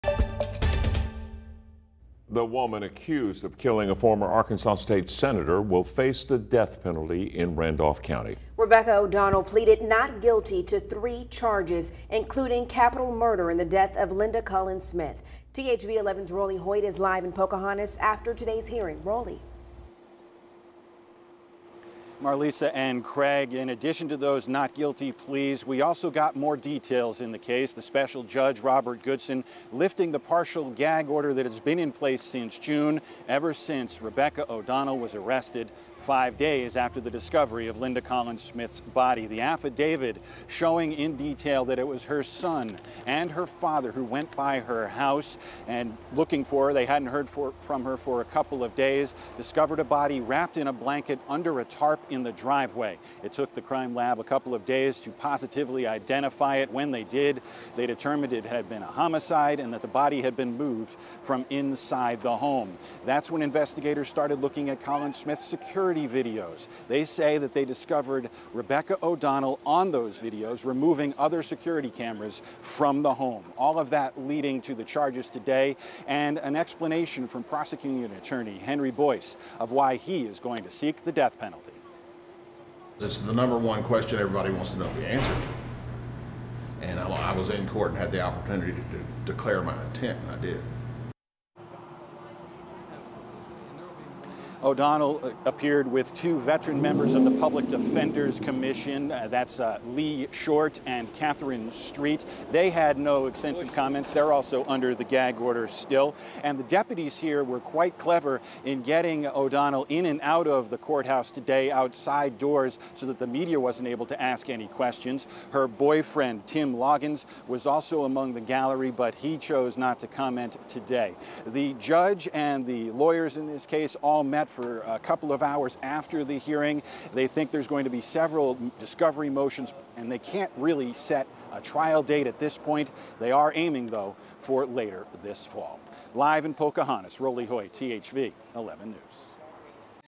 Rebecca O'Donnell facing death penalty in Collins-Smith murder case
Details are becoming clear in the murder case of former Arkansas State Senator Linda Collins-Smith. The woman accused of the crime is facing the death penalty.